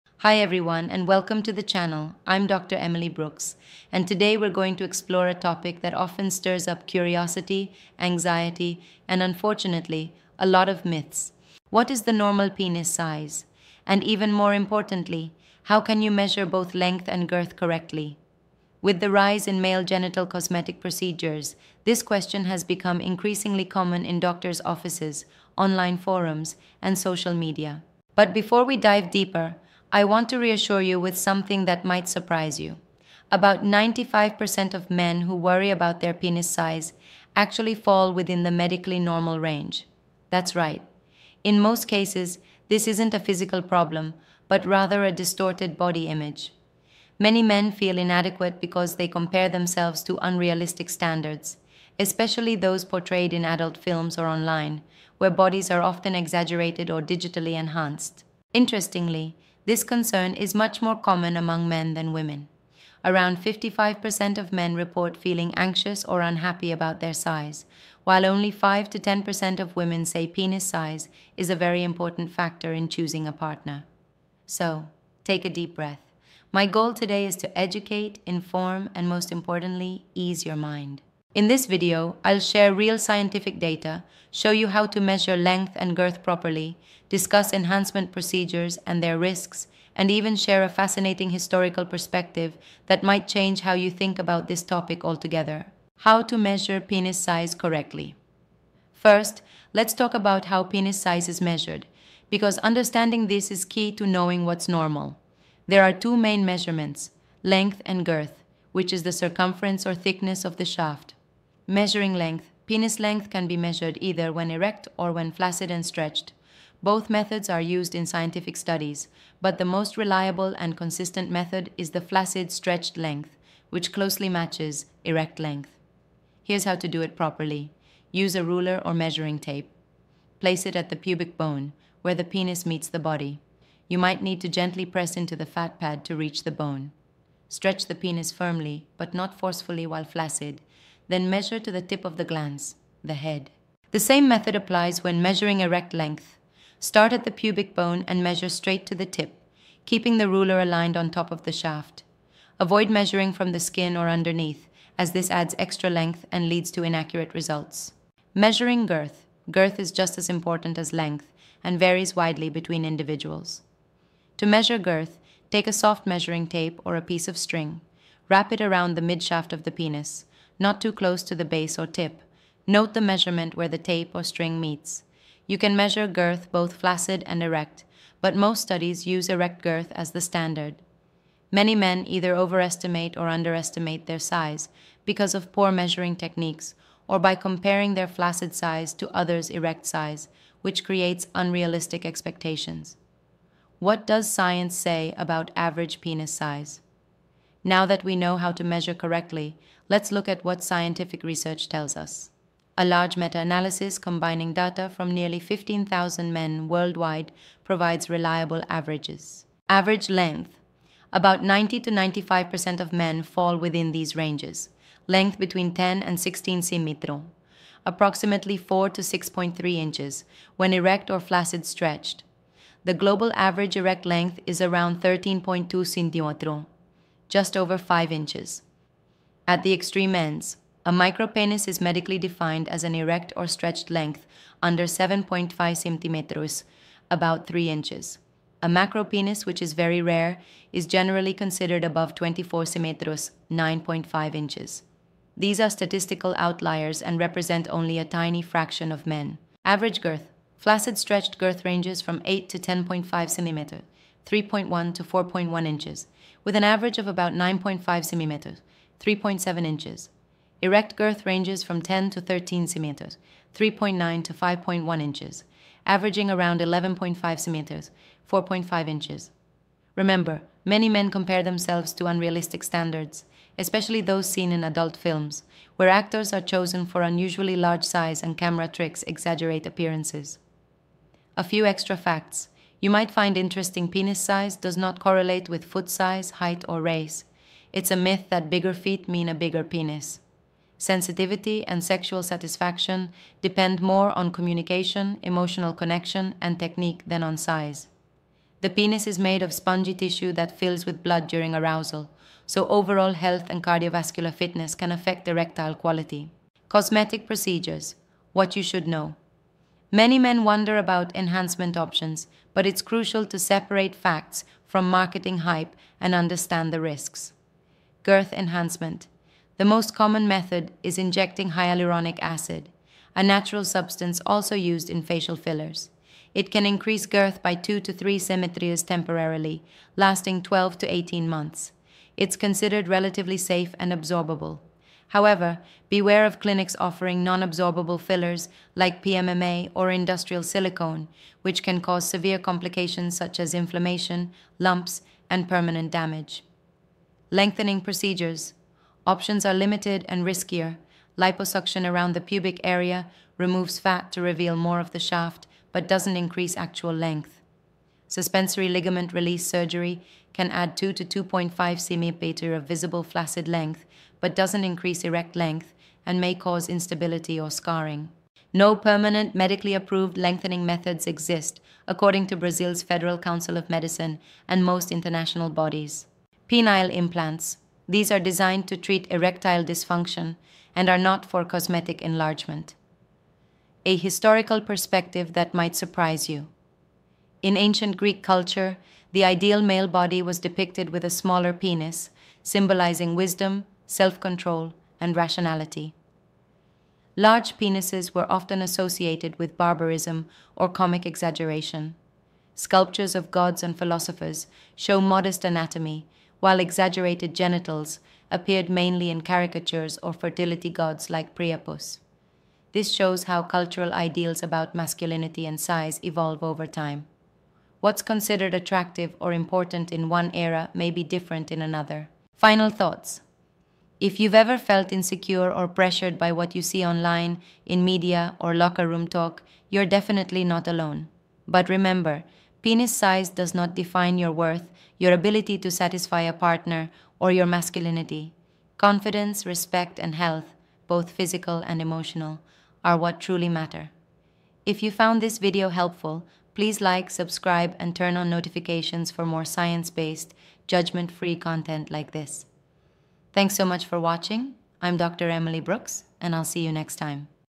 What is the Normal Penis Size? How to Measure Length and Girth Accurately? | UROLOGIST EXPLAINS
What’s the Average Penis Size? The Science, Myths & Measurement Guide for Men

In this informative video, we explore one of the most common — and often misunderstood — questions: What is the normal penis size? With a respectful and evidence-based approach, Laura breaks down everything you need to know about average penis length and girth, measurement techniques, and body confidence.

🔍 What You’ll Learn:

✅ How to measure penis size accurately — length & girth
✅ Scientific data from a major meta-analysis of 15,000+ men
✅ Differences between flaccid, stretched, and erect size
✅ The real definitions of micropenis and macropenis
✅ Insights into penis enlargement methods — what’s safe and what to avoid
✅ Historical and cultural perspectives that may shift your mindset

📊 Based on trusted sources like:
– British Journal of Urology International (BJUI)
– Veale et al. Meta-Analysis (2015)

💬 Whether you're curious, anxious, or just want clarity, this video offers honest, reassuring, and science-backed information to help you understand your body better.